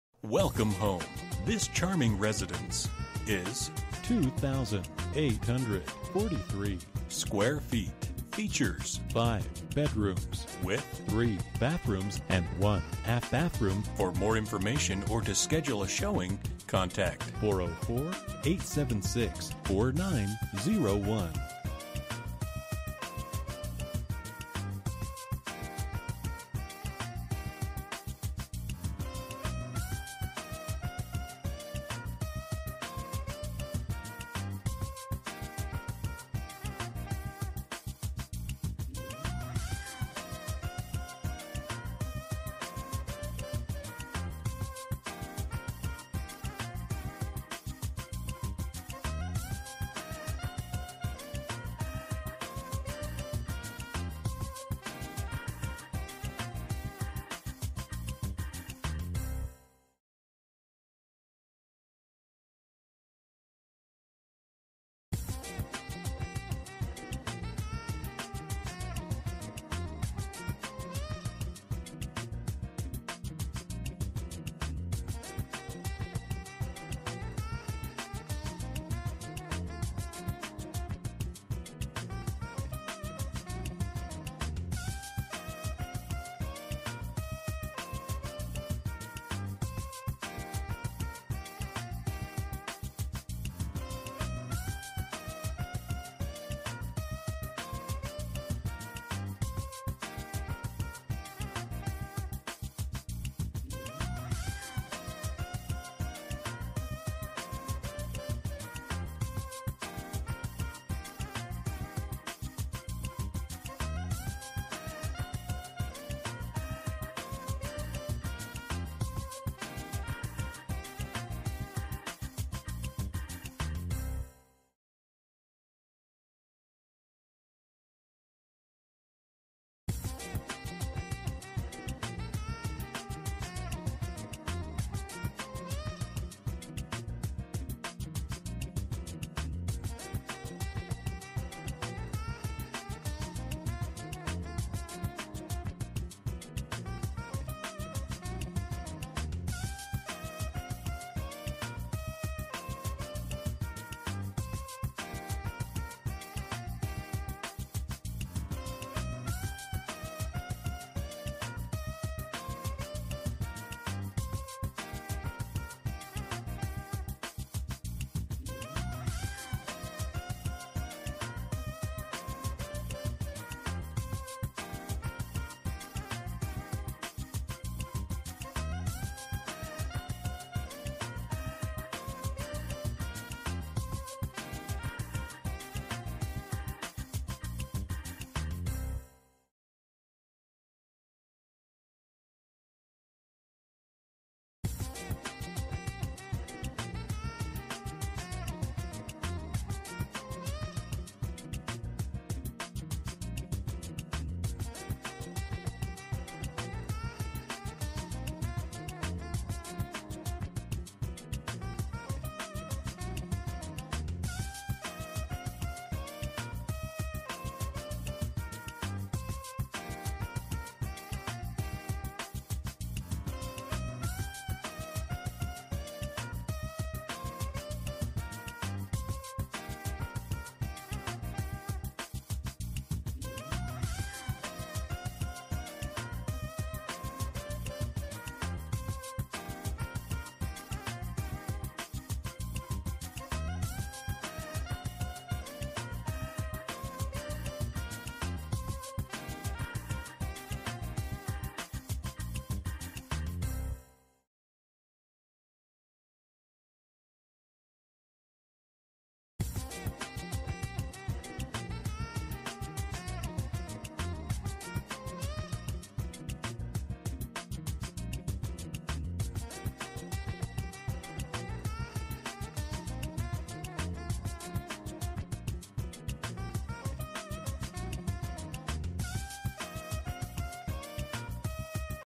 Residential for sale - 3147 Overlook Hill Pass, Dacula, GA 30019
IMMACULATE 5 BEDROOM 3.5 BATH HOME W/LARGE PRIVATE BACKYARD. NEW HARDWOODS ON 1ST FLOOR, LIVING RM, DINING RM & FULL BASEMENT W/SEALED FLOORS. THIS HOME RESTS ON CUL-DE-SAC & BOASTS A NEUTRAL BRICK FRONT EXTERIOR,EAT-IN KIT W/GRANITE COUNTER TOPS COMPLETE W/APPLIANCES AND OPEN TO FAMILY RM W/FIRE PLACE. UPSTAIRS OFFERS HUGE MASTER W/SITTING RM & WALK-IN CLOSET, 4 ADDL BEDROOMS PLUS GENEROUS LAUNDRY ROOM. THIS WELL MAINTAINED BEAUTY IS MINUTES FROM MALL OF GA, Hwy 316 AND I-85. DONT MISS THIS ONE!

Bedrooms: 5
Bathrooms: 3 full and 1 half
Square Feet: 2843
Price: $329,997